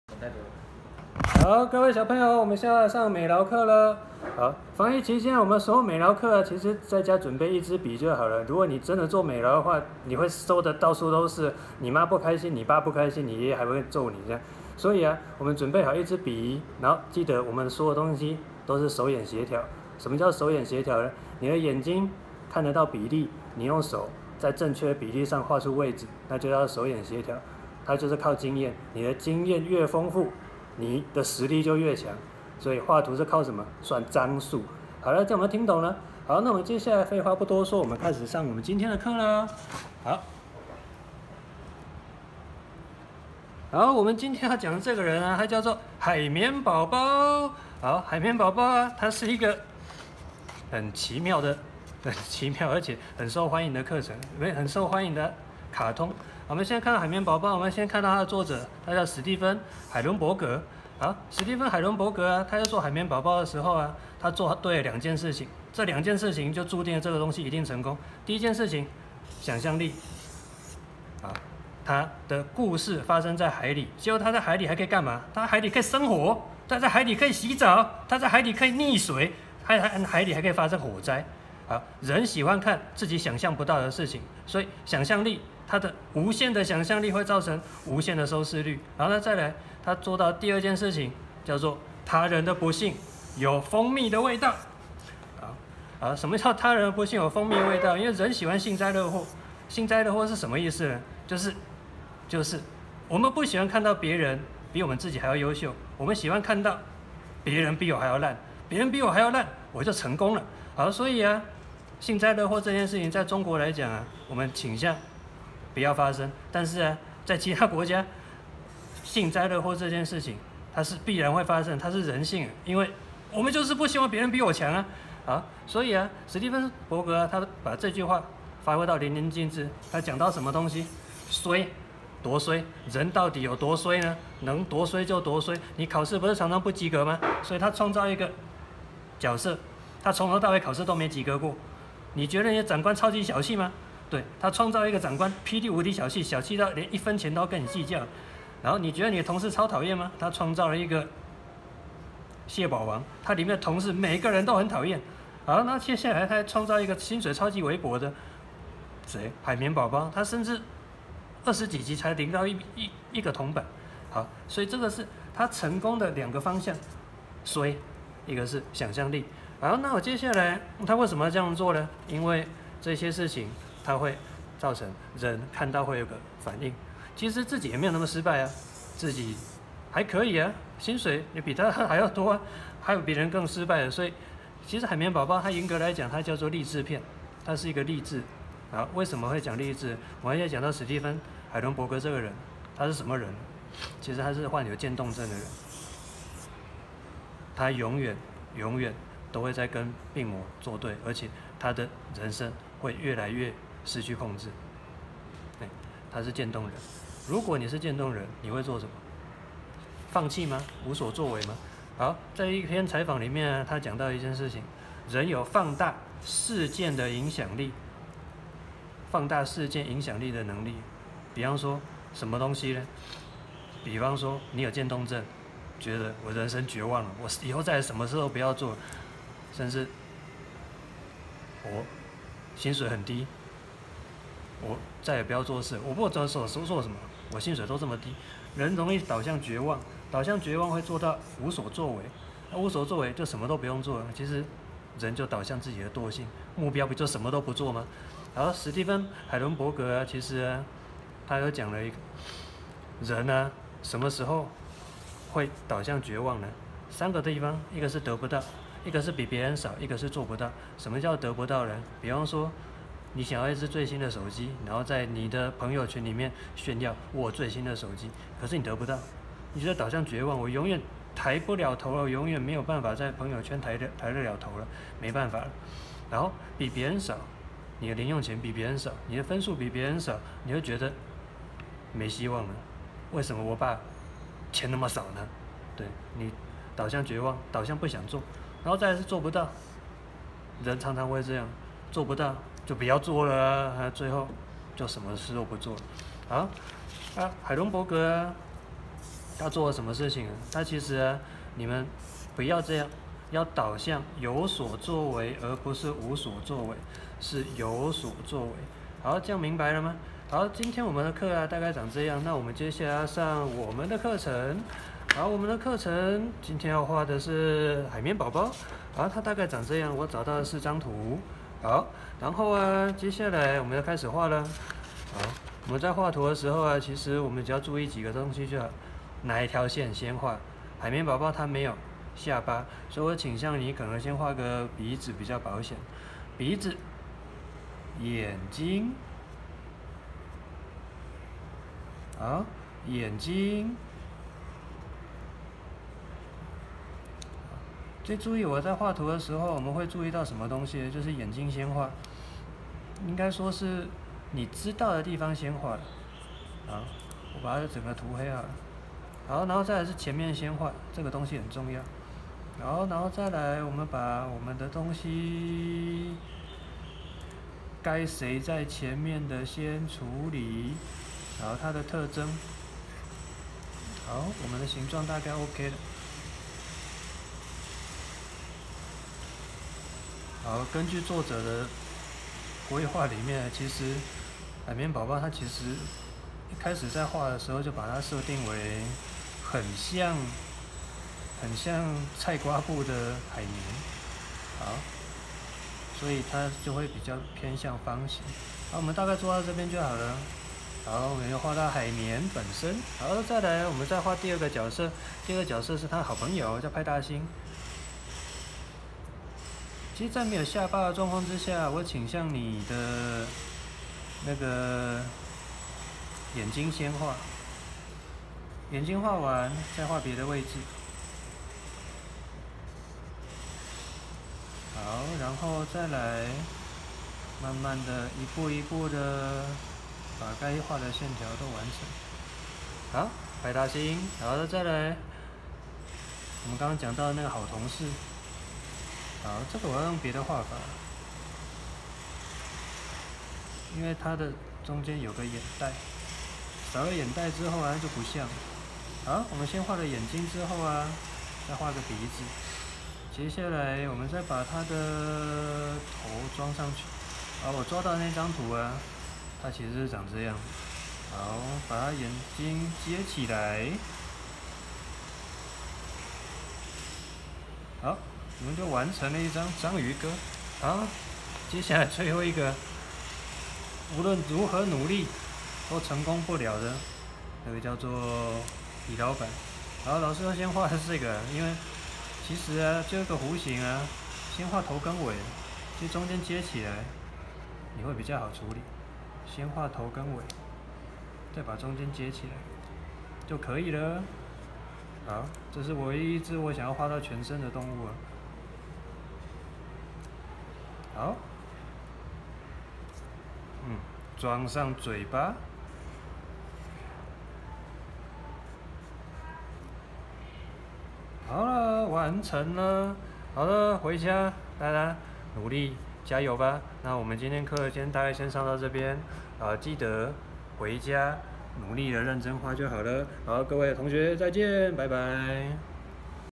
如何畫海綿寶寶 防疫期間，一枝彩色筆的線上美勞課，來自美濃國小。他人的不幸，有蜂蜜的味道
2017年3月13號，史蒂芬發表聲明自己罹患肌萎縮性脊髓側索硬化症（Amyotrophic lateral sclerosis，即 ALS），俗稱「漸凍人」，但他也表示會繼續為大家創作，不輕易放棄。史蒂芬發表聲明：「任何認識我的人都知道，只要我還可以，我就會繼續創作《海綿寶寶》以及追隨我其他的熱情。」然而一年半後，史蒂芬仍然不敵此症所引發的心臟衰竭，而於2018年11月26日在他位於加州聖瑪利諾的家中逝世，享年57歲。
(維基百科)